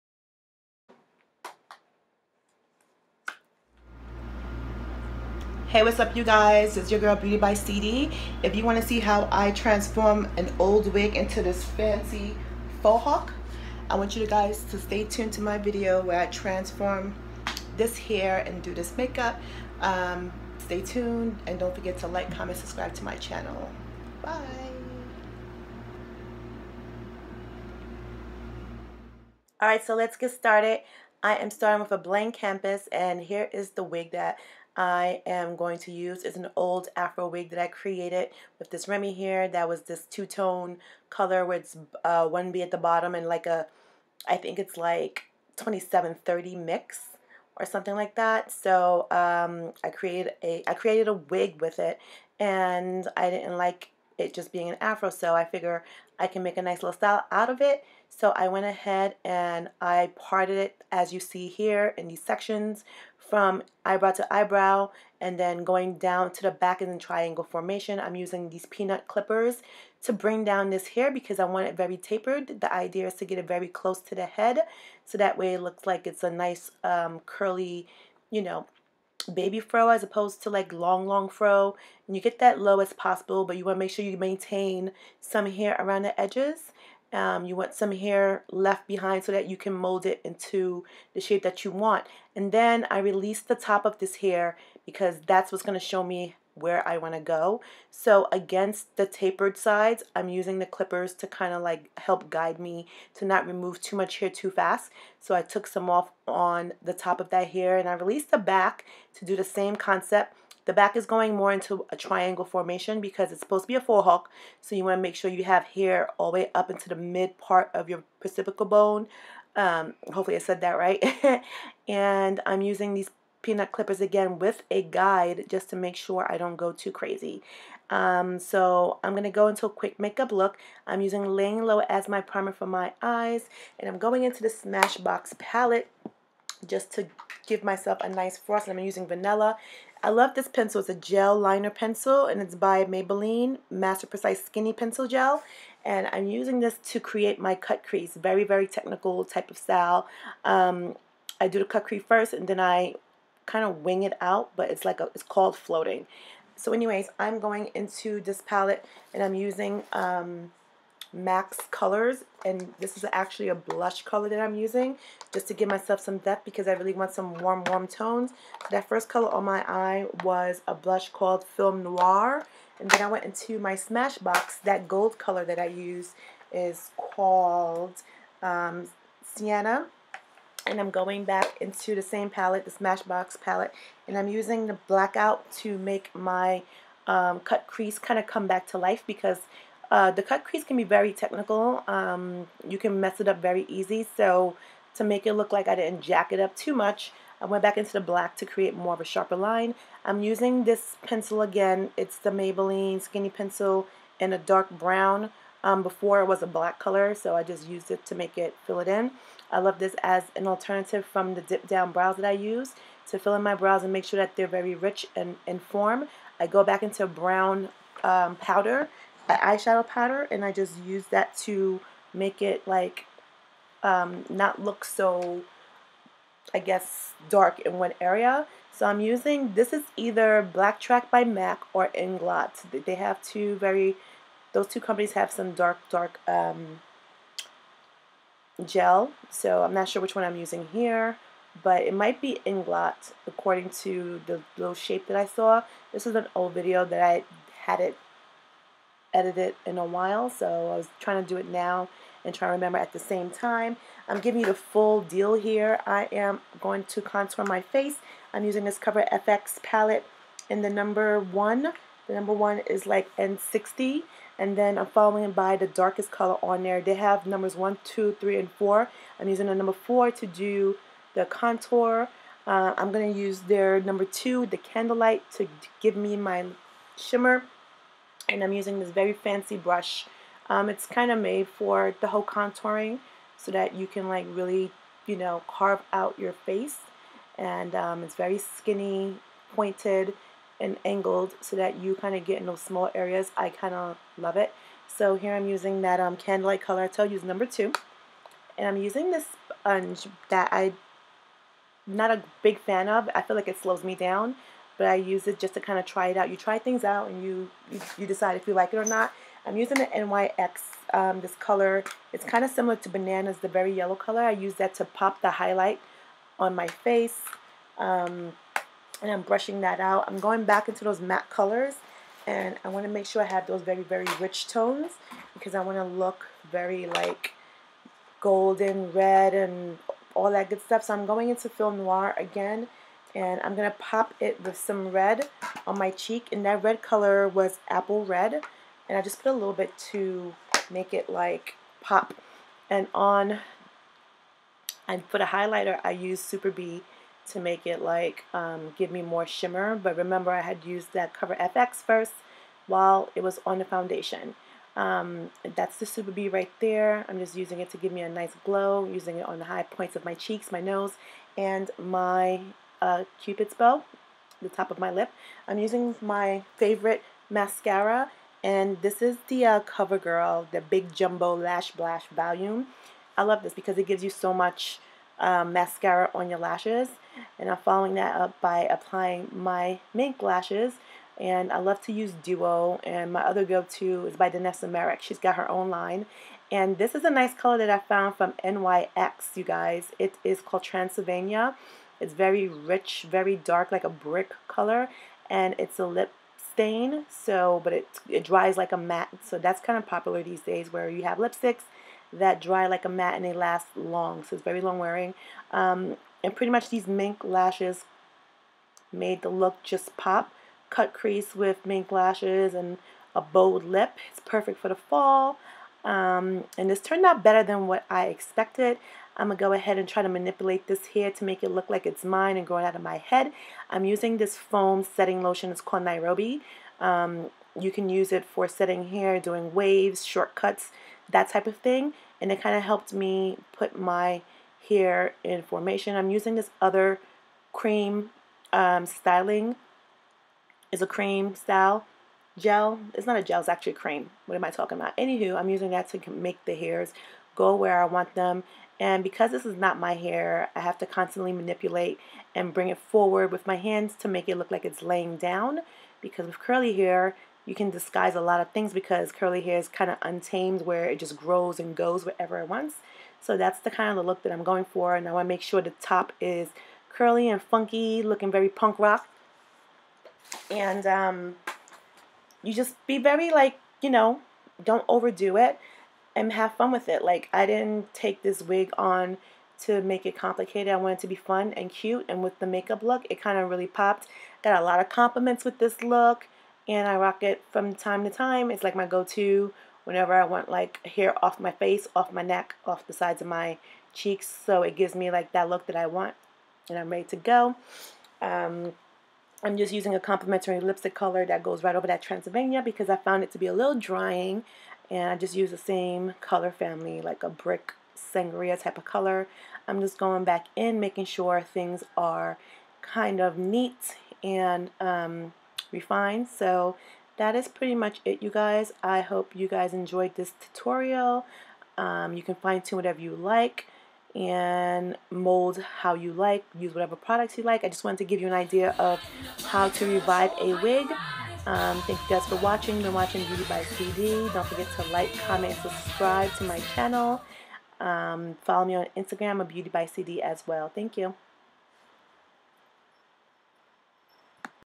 Fohawk Mohawk SLAYAGE
Hey welcome back .. this is going to be a quick video on how to relive a afro wig and turn it into something dope and useable.   Im going to show you how i transform an old wig into a edgy mohawk .You will also get the makeup  for this look -Enjoy

details
foundation Nars
gel pencil - maybeline skinny gel pencil in black and brown
blush colors used for eyes and face is Film Noir by Mac and Red
LipStick by NYX - Transylvania
smash box colors on the eyes are sienna for the gold looking color and blackout for the black
contour - cover fx kit - numbers 1, 2, 4
nyx -highlight powder in cream
mascara - cover girl lash blast
liner- ingot in black
mink lashes
glow shimmer - mac skin finisher - in superB